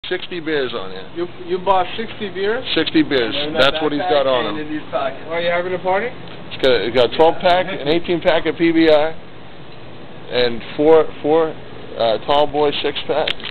The Legend of 60
this kid came into sleven and bought 60 beers and left on foot no problem.

Travis Curran tasty dudes

the box

Mohammed YAQUB